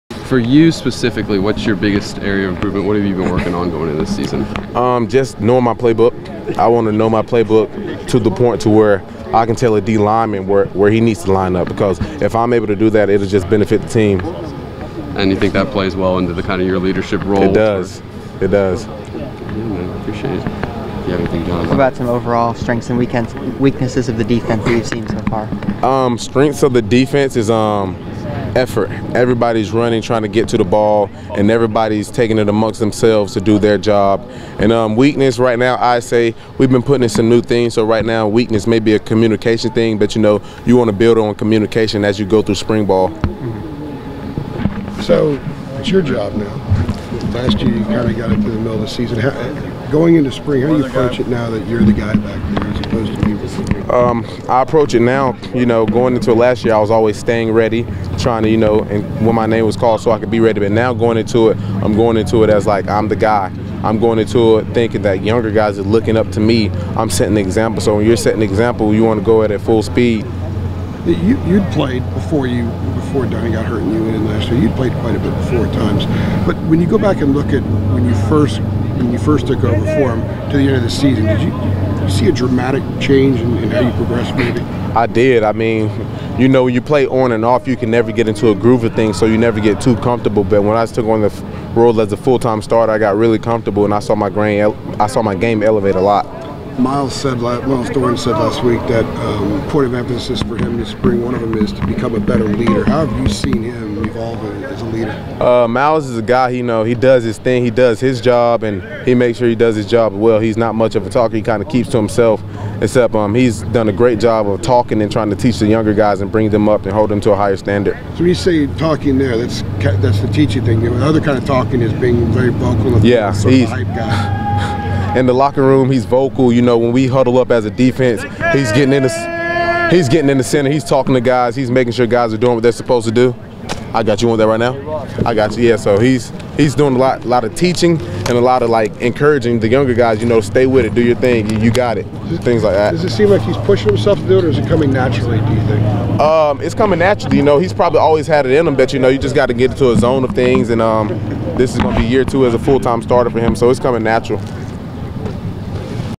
ICTV: J.K. Britt on Strengths of the Defense
The rising senior safety is leading the unit during spring practices.